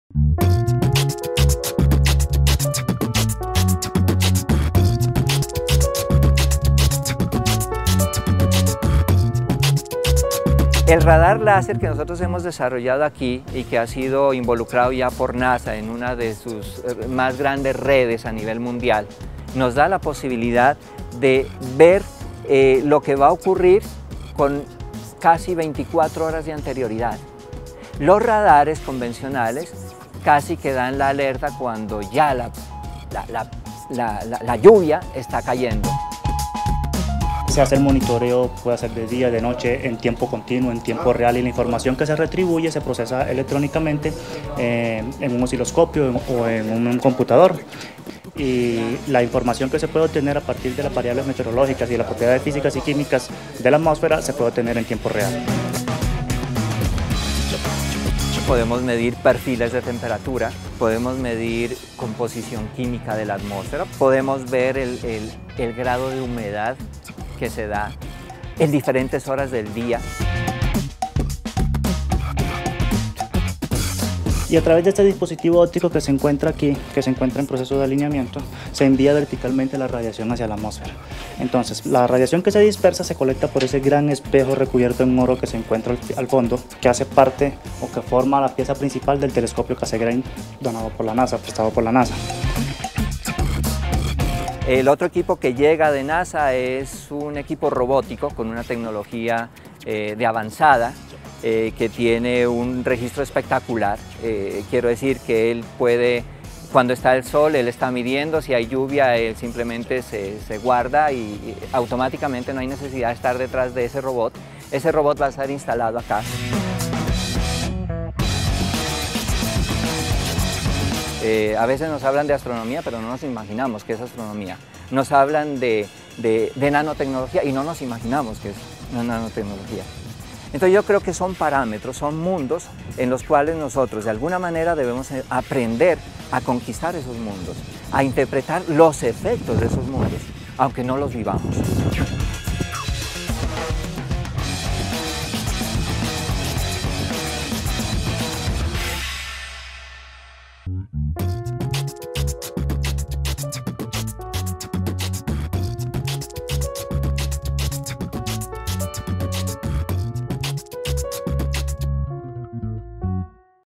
NASA en Medellín
En la Universidad Nacional sede Medellín, está un equipo encargado de trabajar con láseres y espectroscopia, que con apoyo de la Nasa está haciendo investigaciones muy tesas y que aspira pronto convertirse en una sede del ente espacial en nuestra ciudad para investigar el por qué de los cambios climáticos en esta región.